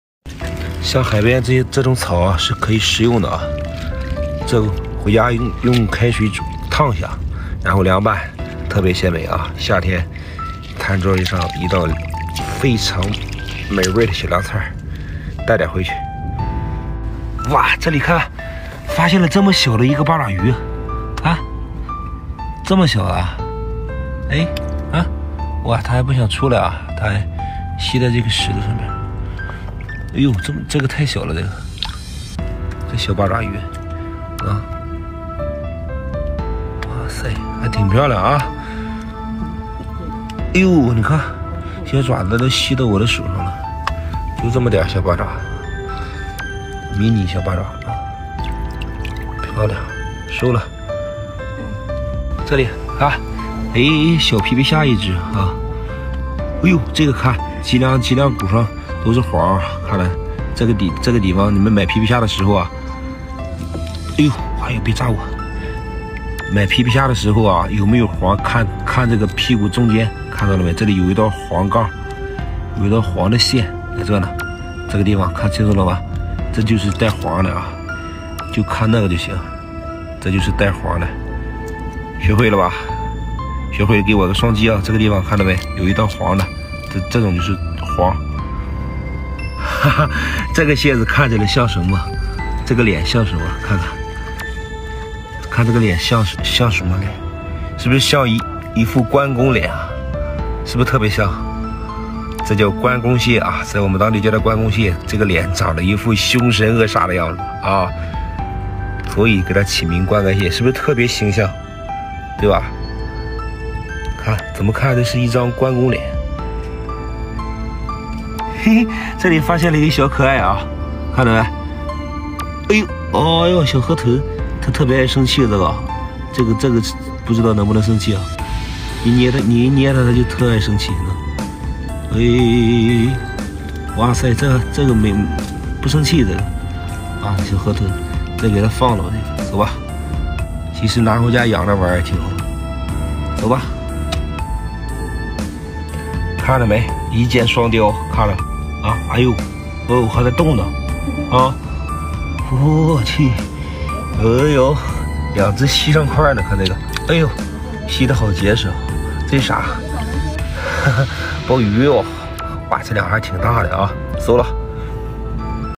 Catching Seafood 🦀🐙 ASMR Relaxing (Catch Shark , Catch Fish ,Deep Sea Monster ) - Tik Tok #360
@Copyright by 1-Minute Creatures
@Please do not Reup